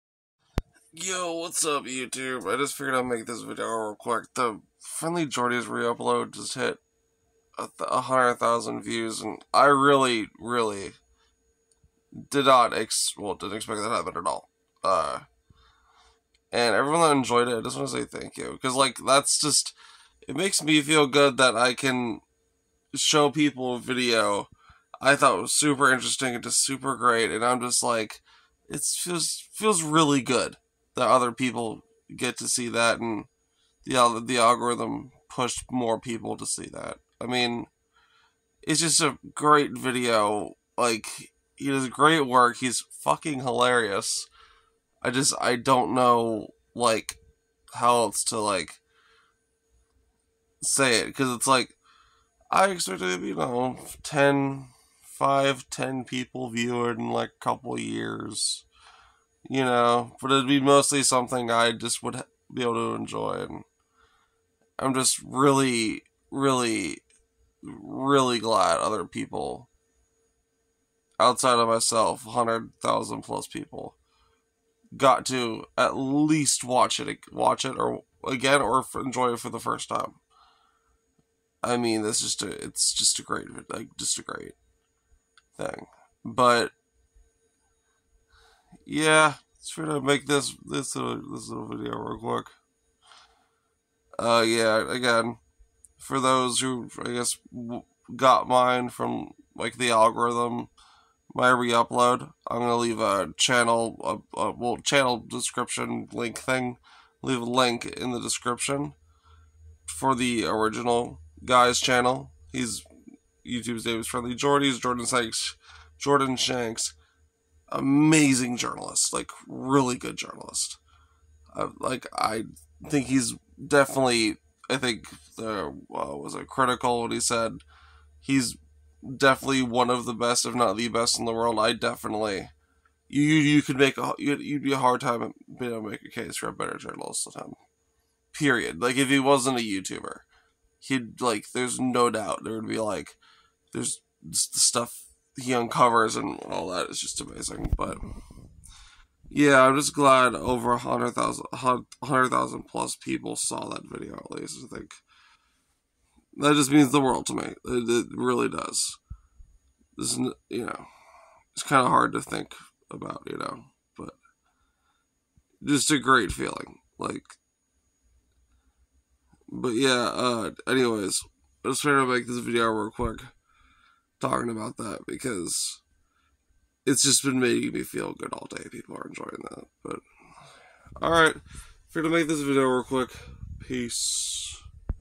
my @friendlyjordies reupload got 100k views!!!!
I'm just super happy 100k+ people viewed that ‎@friendlyjordies  reupload. I didn't expect this many people to see it. I'm gonna leave a link to his channel.